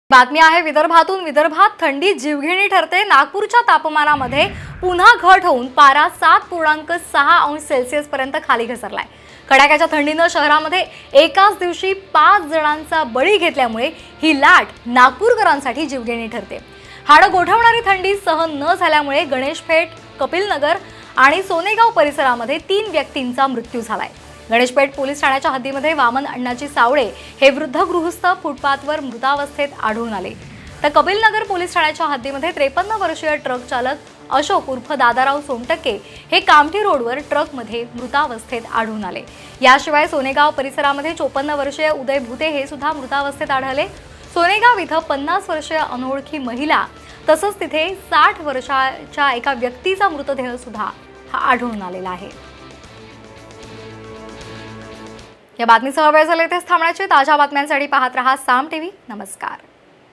Nagpur Winter News | उपराजधानीत थंडीचे पाच बळी | Five victims of cold in Vidarbha | Sakal Media
विदर्भात जीवघेणी थंडी. उपराजधानीत थंडीनं घेतले पाच बळी.थंडीमुळं चौघांचा रस्त्यावर मृत्यू. (Five victims of cold in Vidarbha)